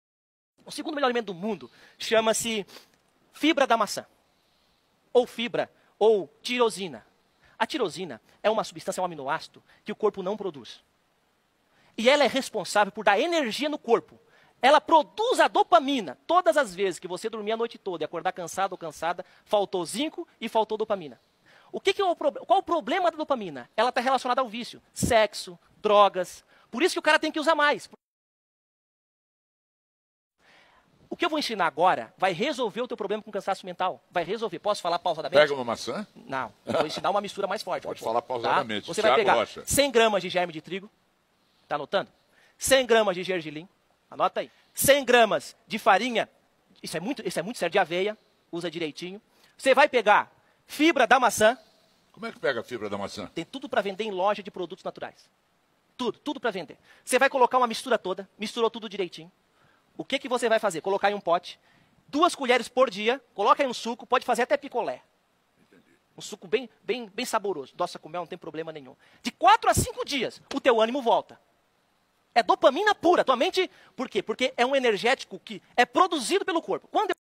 BOMBA DE DOPAMINA
A fibra da maçã é importante para fornecer energia ao corpo, pois contém tirosina, um aminoácido responsável pela produção de dopamina. A falta de zinco e dopamina pode causar problemas de fadiga e falta de energia. O cansaço mental pode ser resolvido através do consumo de uma mistura contendo gérmen de trigo, gergelim, farinha de aveia e fibra da maçã. Essa mistura pode ser consumida diariamente, adicionada a sucos ou mesmo utilizada para fazer picolés até saborosos. Ao consumir essa mistura, a energia e os ânimos serão restaurados devido à produção de dopamina pelo corpo, em contraste com os energéticos que podem causar depressão após o efeito estimulante.